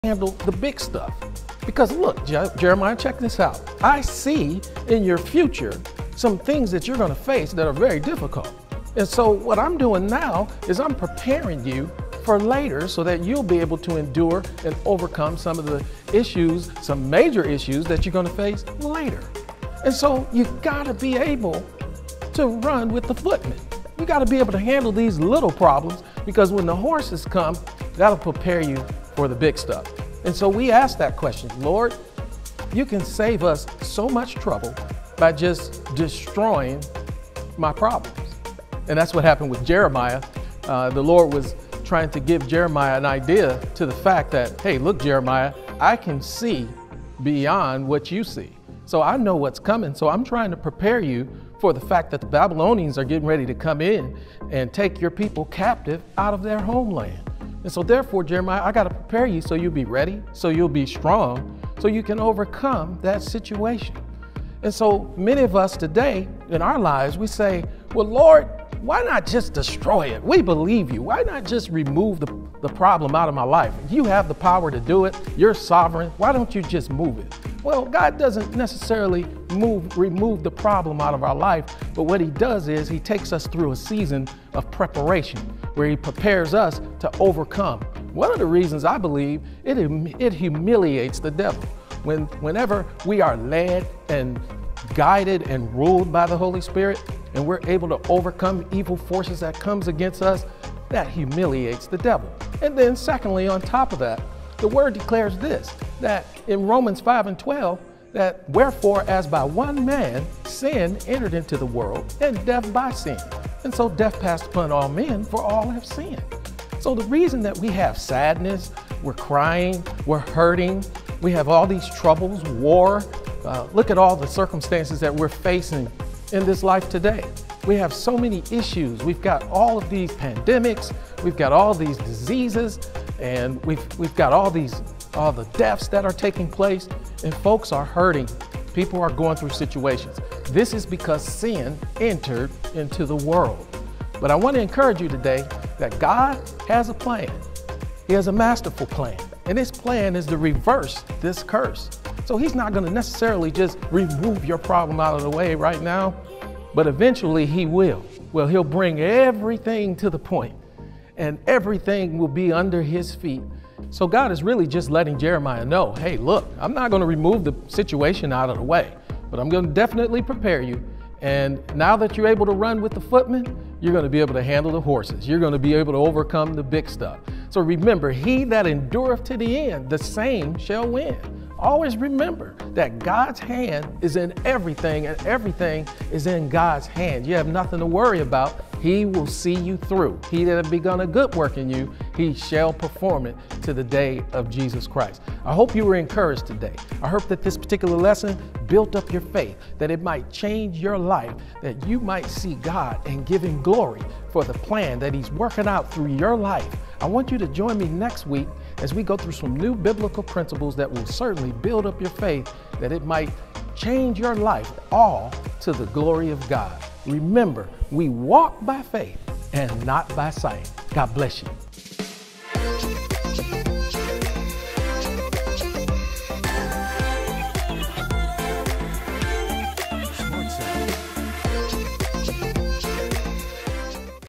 "Can We Run With The Horses"
Small trials prepare you for major trials...Take a listen to this biblical principle!!